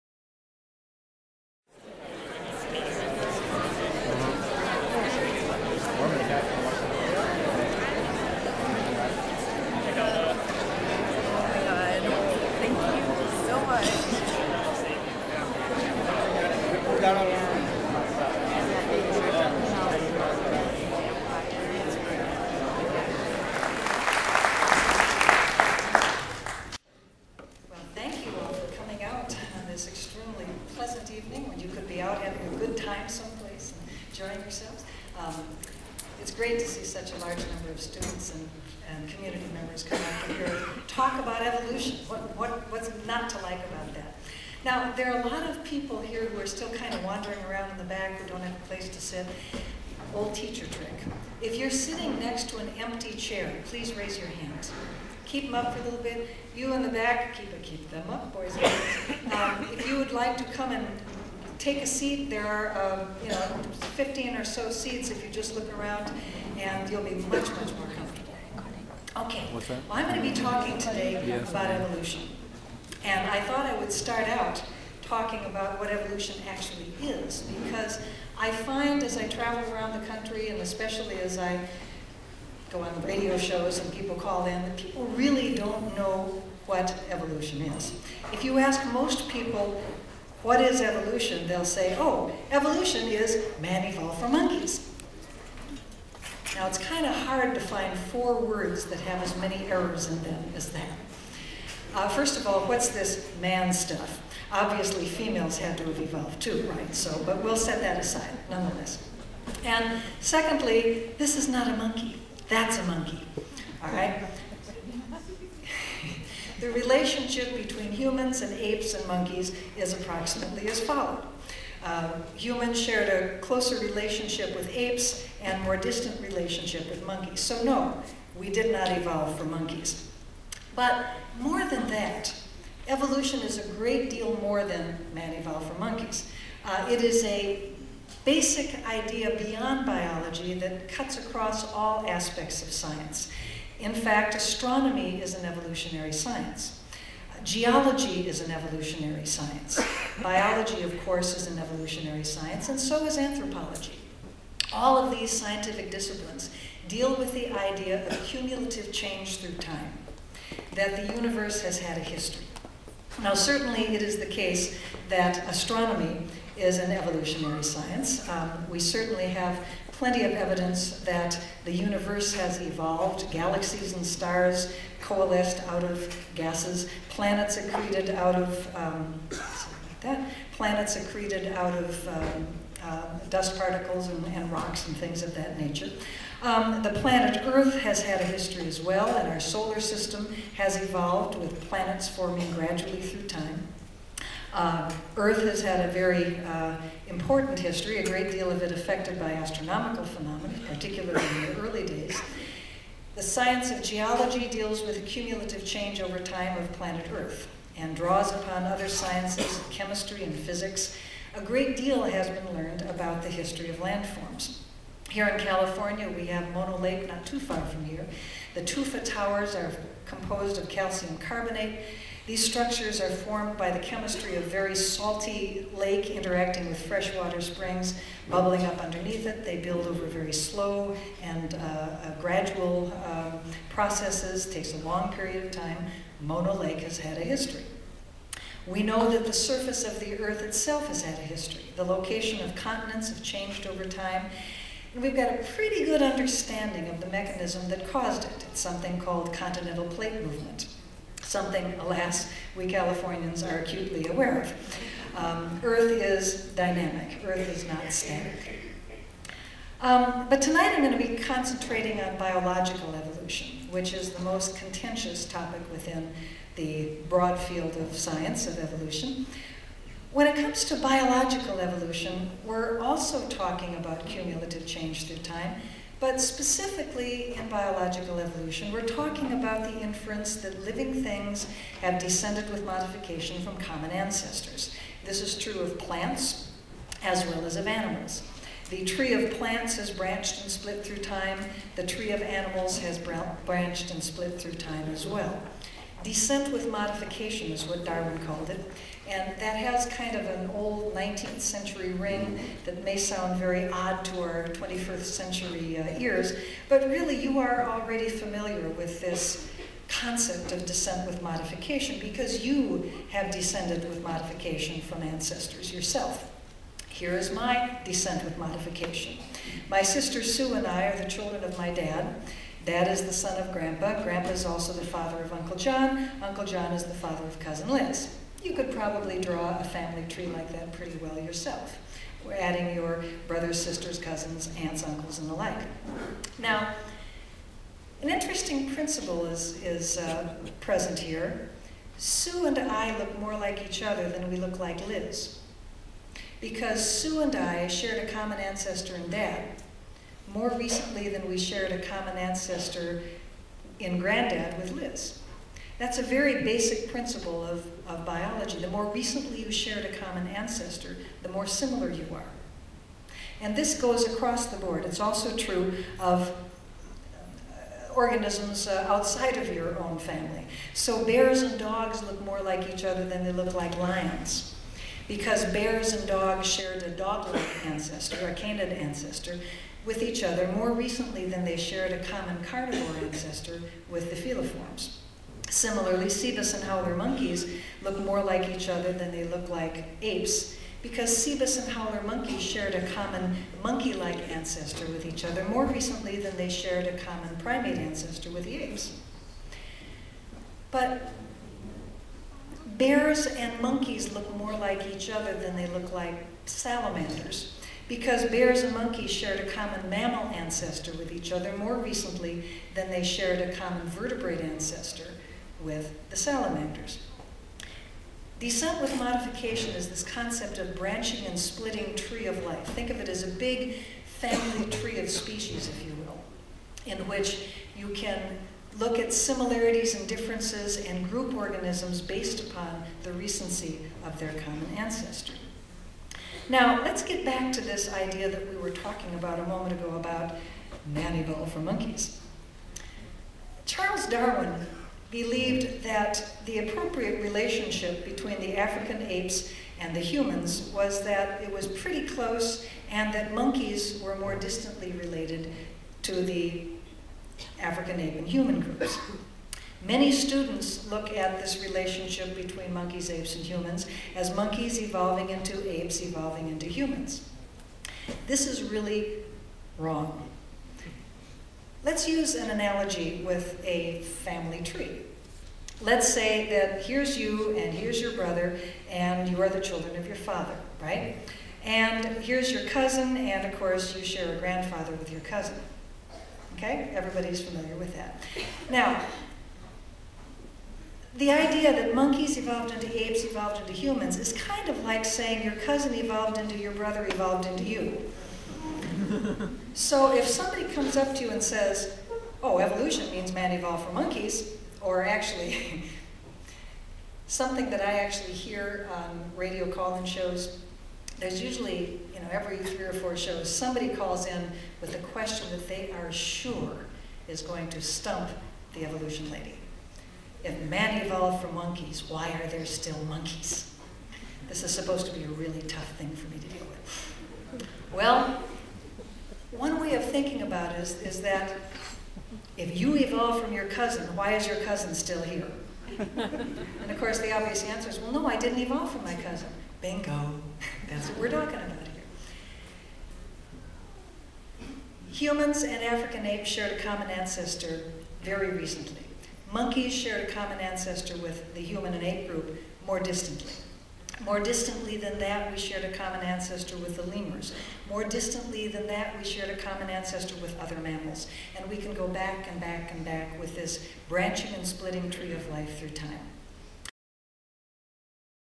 Why all the fuss about Darwin and evolution? Part I
Dr. Genie Scott gives Fresno State students the 411 on how evolution works, from cells to cetaceans, and why creationists continue to attack evolution. Event date: 12/2/2009